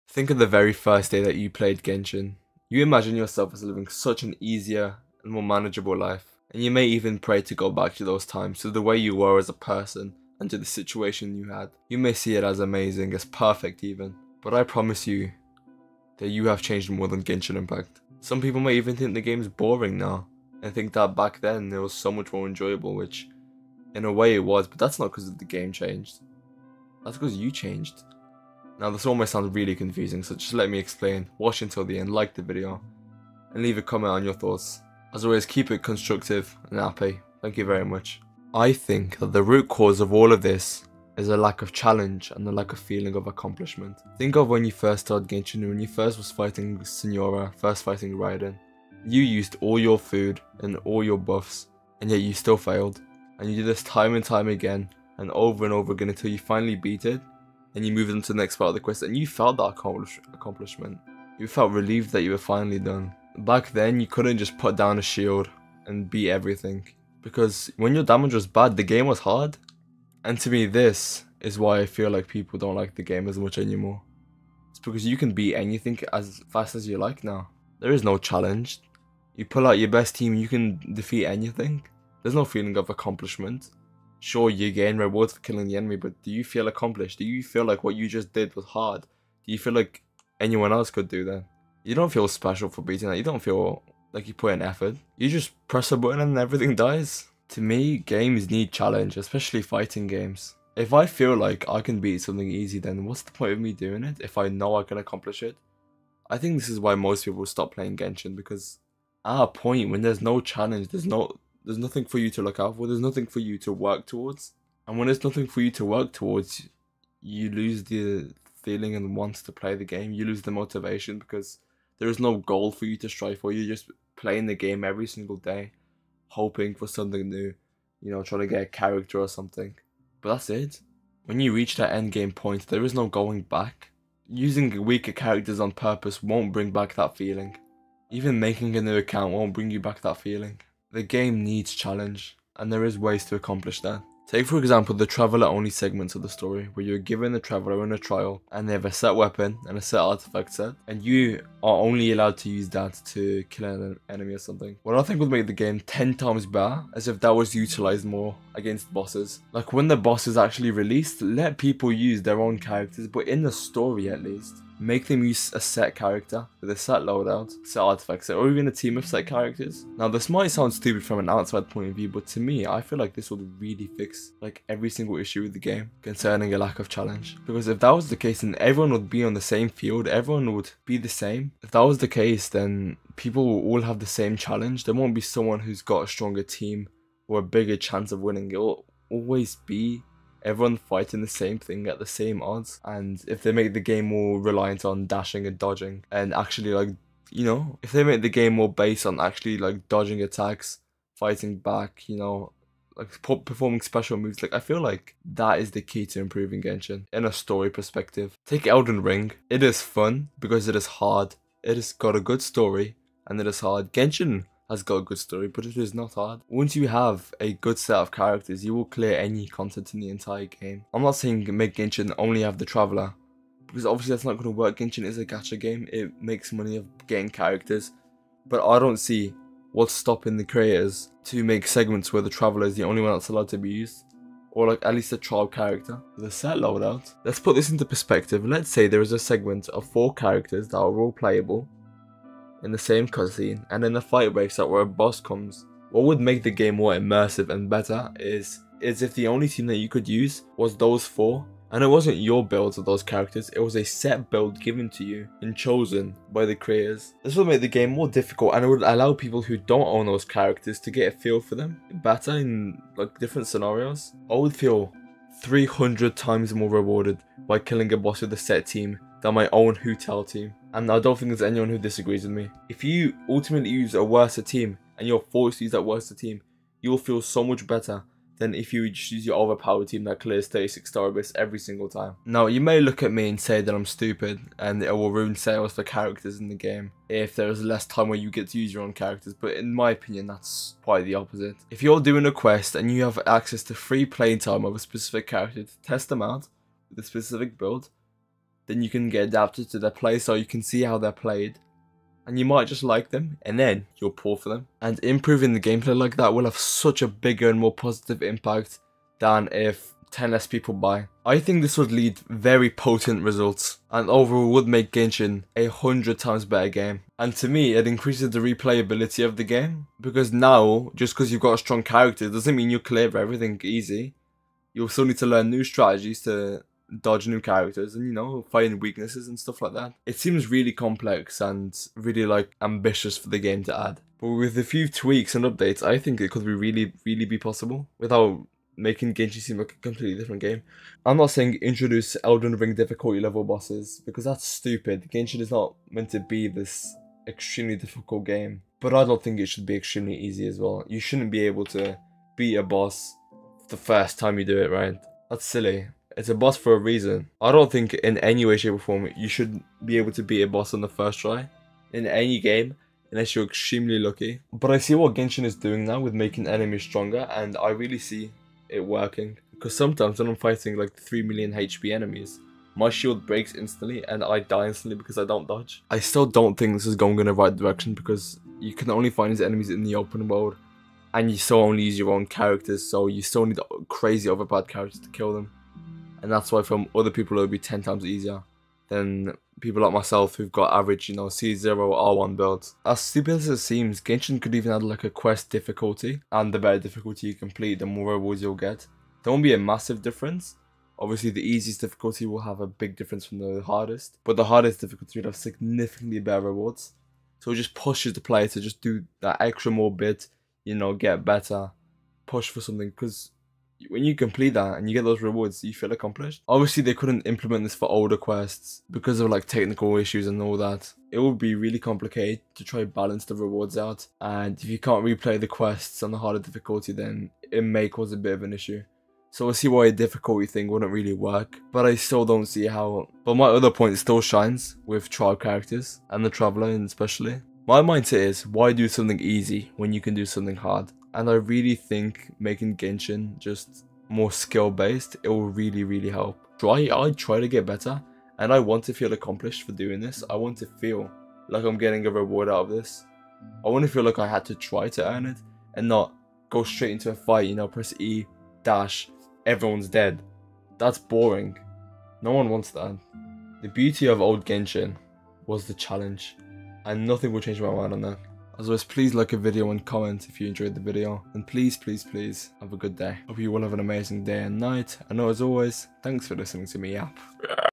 Did Genshin Impact Get WORSE Over Time?
Thanks for looking at my video. I will be answering the question of is Genshin Impact getting worse? And how it connects to personal feelings and how I think those feelings can be replicated.